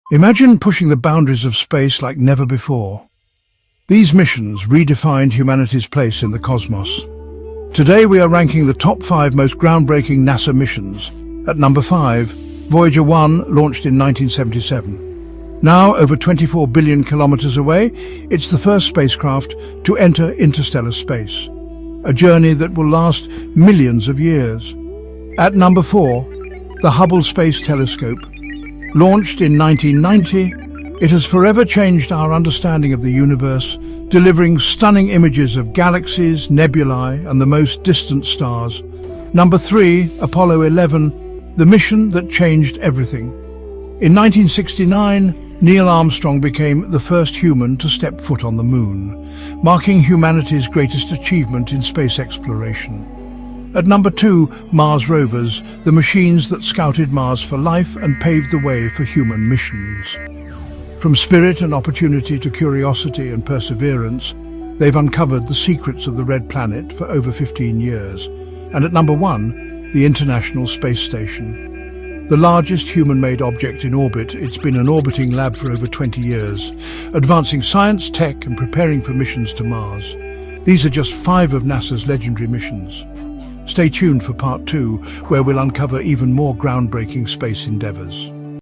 🚀 Top 5 NASA Missions That Changed Space Exploration – Part 1 🌌
NASA has been at the forefront of groundbreaking space exploration for decades, and in this video, we’re diving into Part 1 of the Top 5 NASA Missions that forever changed our understanding of the universe! 🌠 From the Apollo Moon Landings to the Mars Rover Missions, NASA’s achievements continue to inspire and shape humanity’s future in space. 🪐

In this episode, we’ll explore the incredible missions that paved the way for the space exploration of today and the future. Whether you’re a space enthusiast, a fan of history, or curious about the impact of these monumental missions, this video has something for you!

🔭 Missions covered in this video:
1️⃣ Apollo 11 - The first Moon landing
2️⃣ Hubble Space Telescope - Unlocking the universe’s secrets
3️⃣ Mars Rover Missions - Discovering the Red Planet
4️⃣ Voyager 1 & 2 - The farthest-reaching human-made objects
5️⃣ The International Space Station